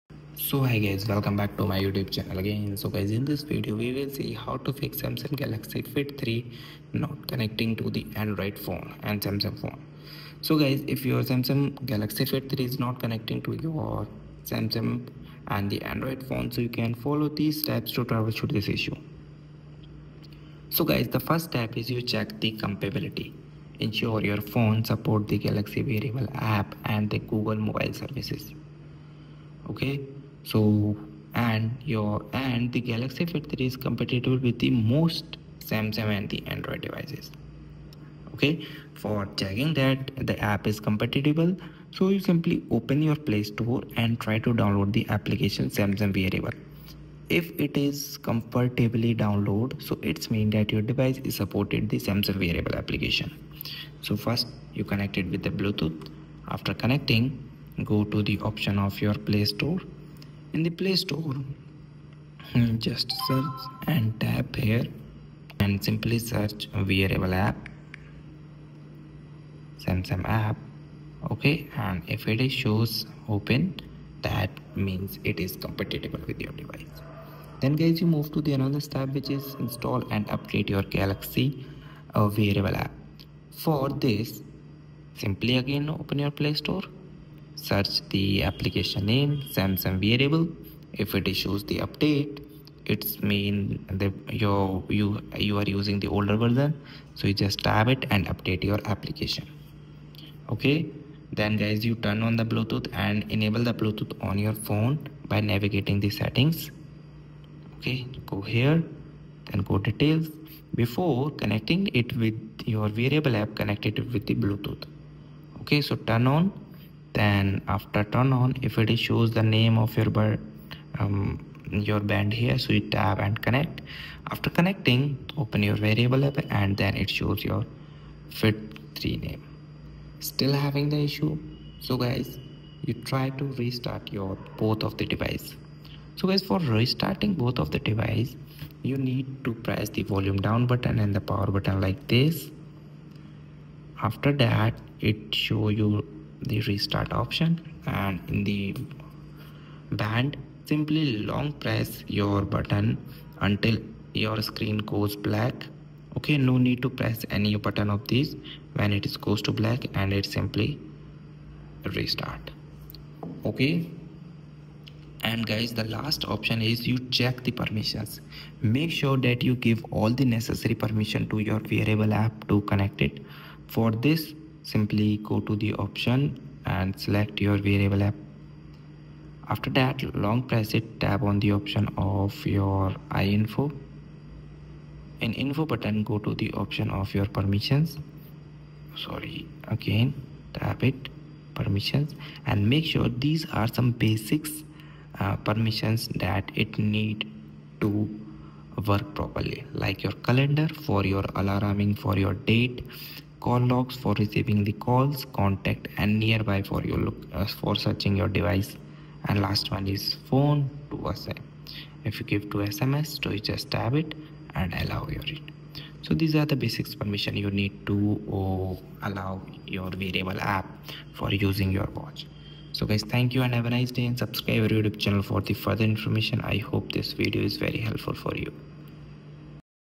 How To Fix Samsung Galaxy Fit 3 Not Connecting To ANDROID Phone [SAMSUNG PHONE]
In this comprehensive tutorial, we will guide you through the steps to resolve the issue of your Samsung Galaxy Fit 3 not connecting to your Android phone. Whether you are experiencing difficulties with Bluetooth pairing or syncing notifications, this video covers all the essential troubleshooting techniques to get your devices connected seamlessly. We will explore common problems, provide detailed solutions, and share tips to ensure a smooth connection between your Samsung Galaxy Fit 3 and your Android device. Don't let connectivity issues hold you back—watch now to regain full functionality!

0:23 Fix 1: Check Compatibility
1:20 Fix 2: Install or Update the Galaxy Wearable App
1:41 Fix 3: Turn on Bluetooth
2:13 Fix 4: Restart Both Devices
2:49 Fix 5: Check App Permissions